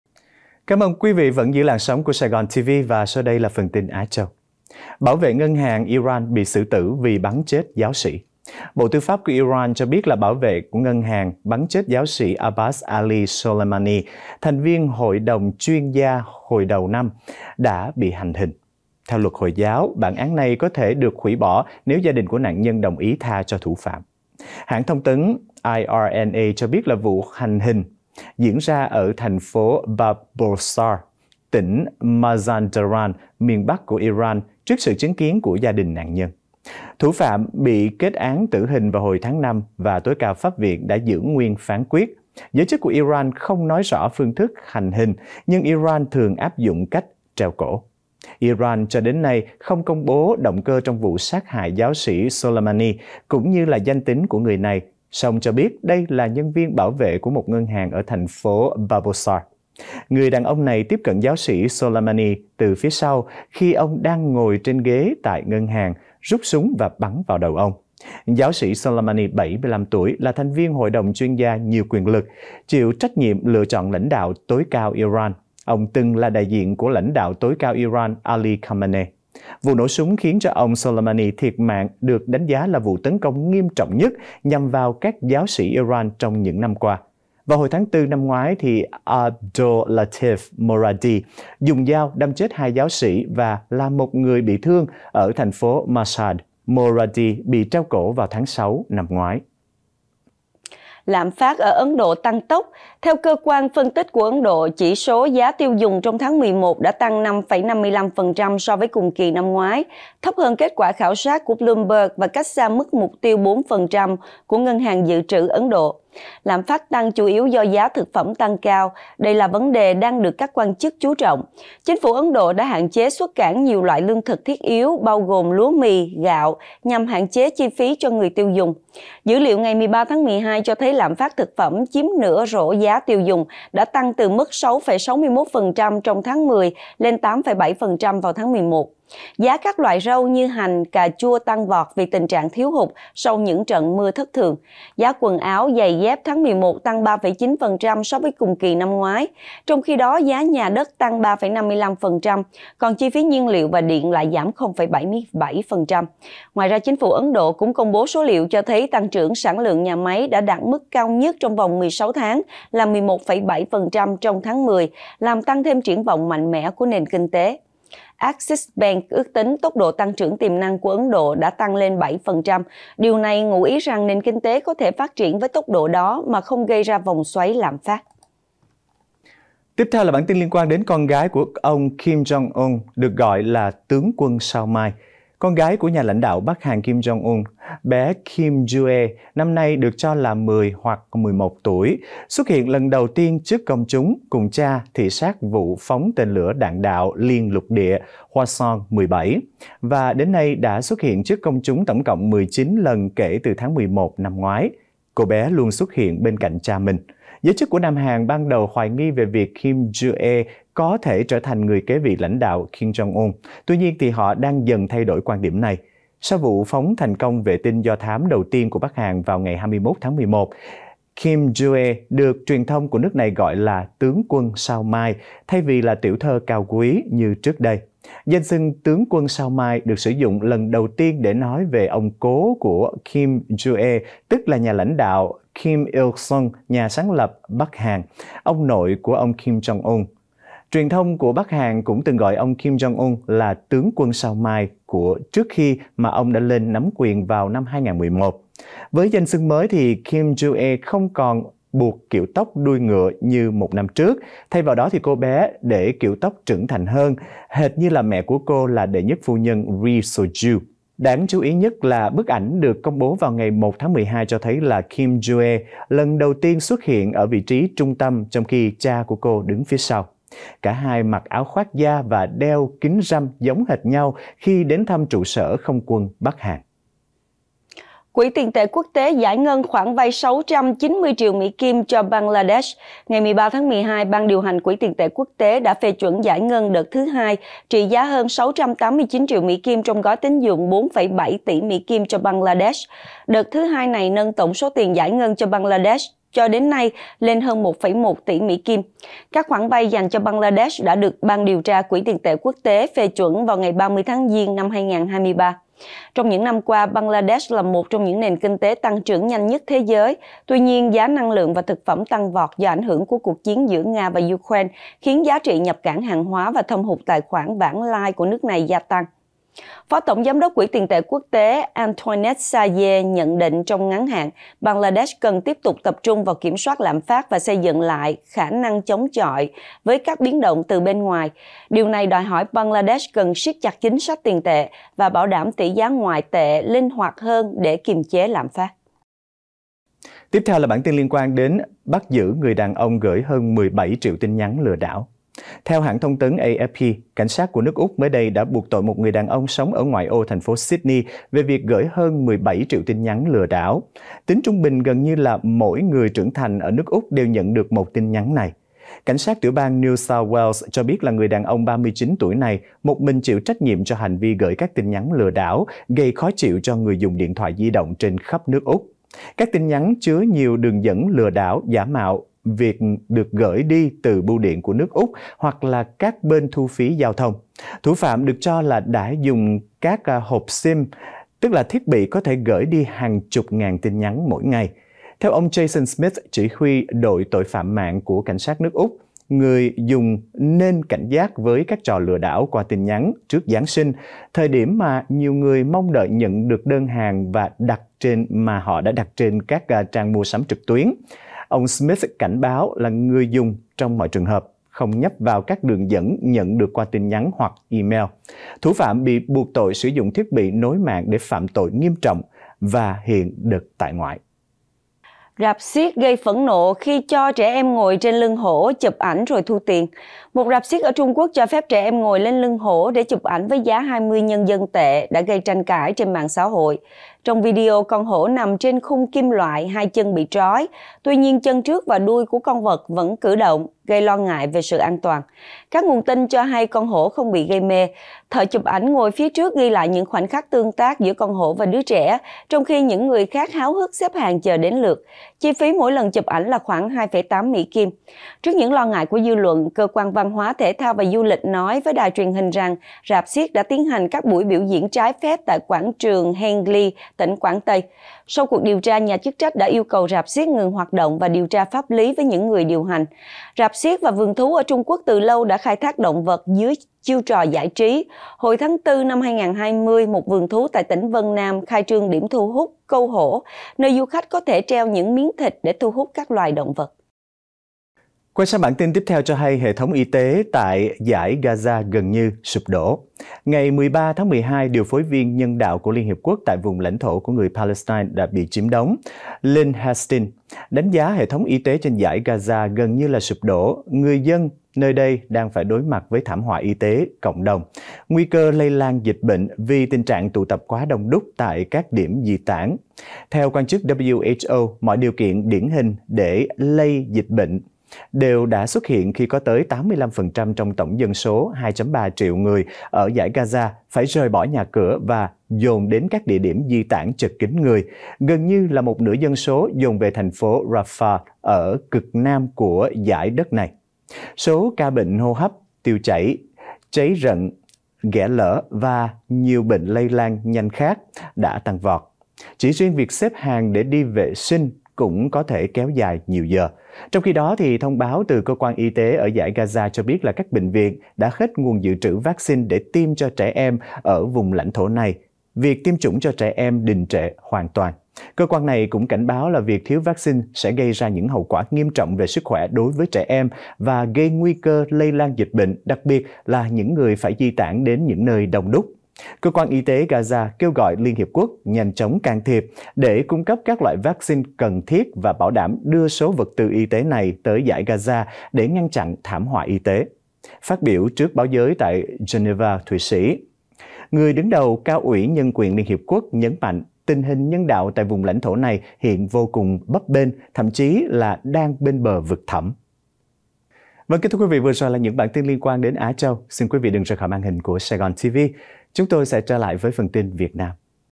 Bảo vệ ngân hàng Iran bị xử tử vì bắn chết giáo sĩ📺Tin Sáng 12/14 | Thời Sự Á Châu
SAIGON TV📺